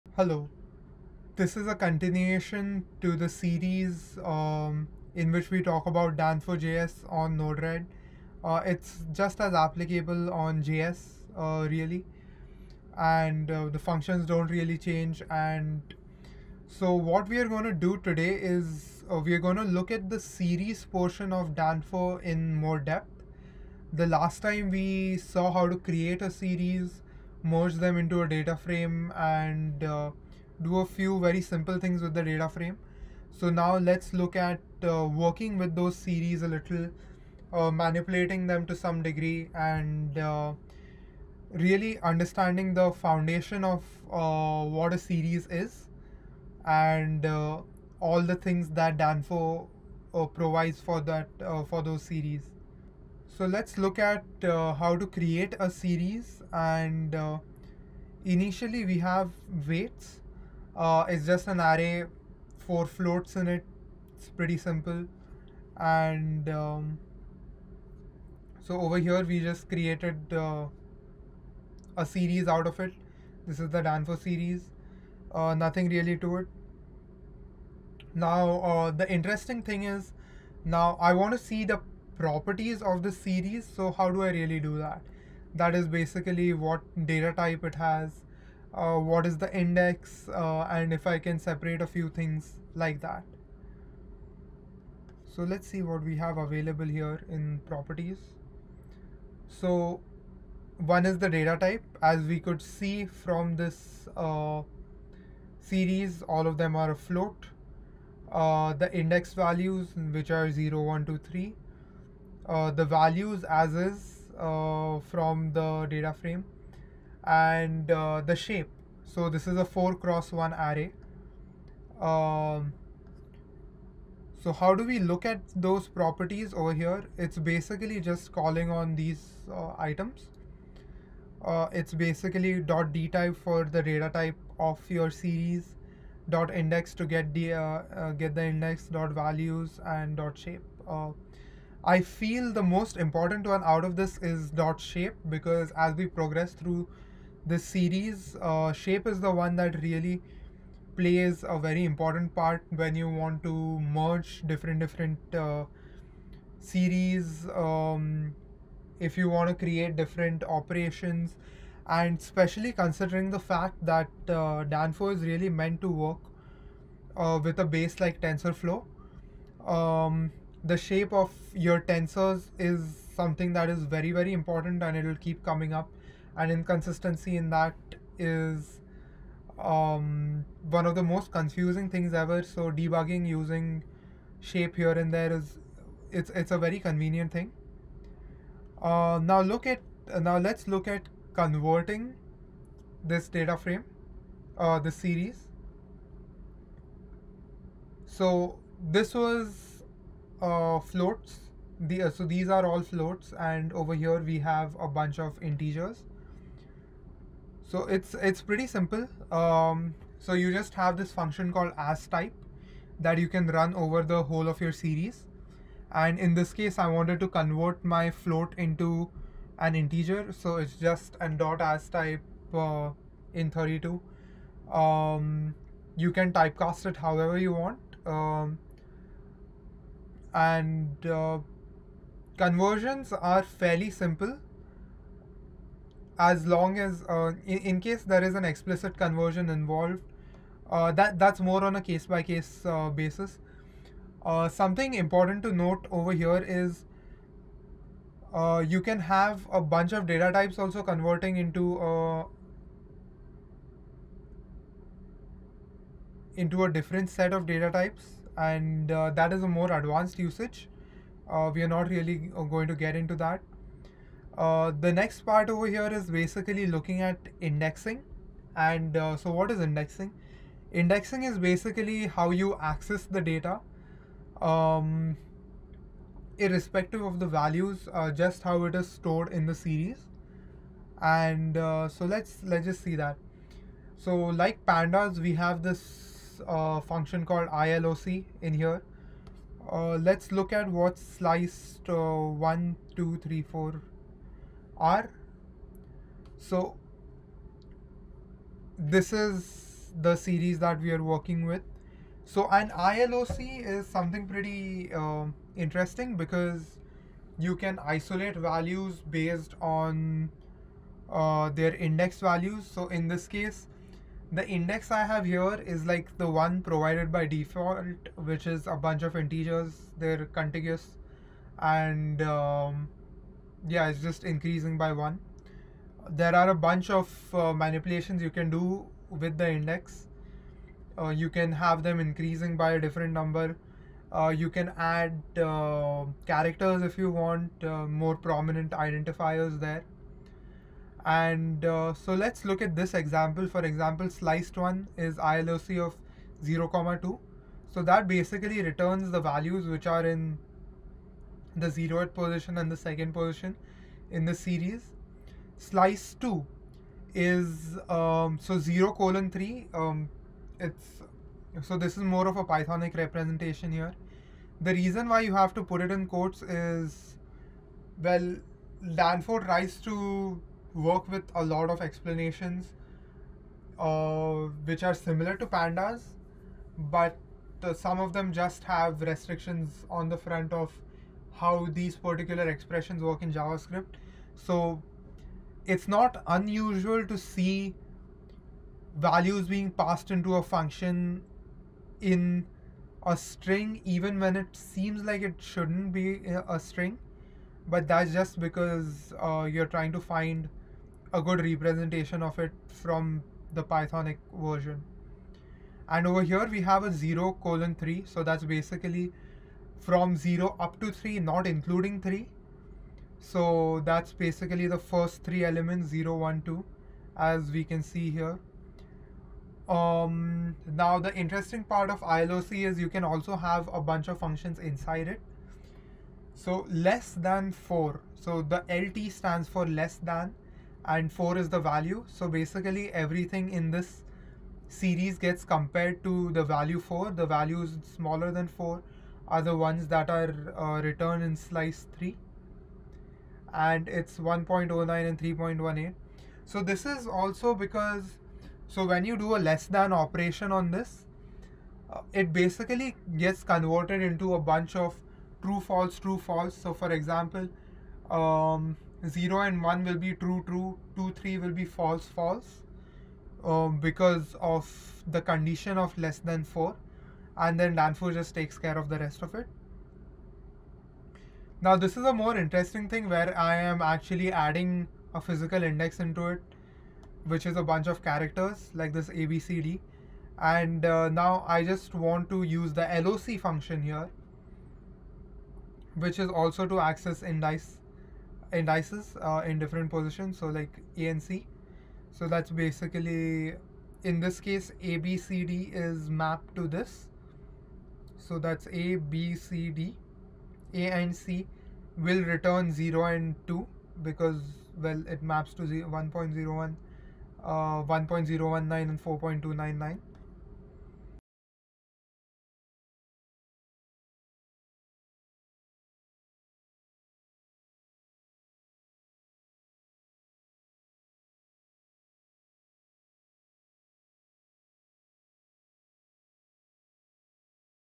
Prescient: PART 03 | Process Data Using Danfo.JS with Node-RED
In the third part of this tutorial, Kartik will take you through how to use Danfo.js to process data in Prescient Designer.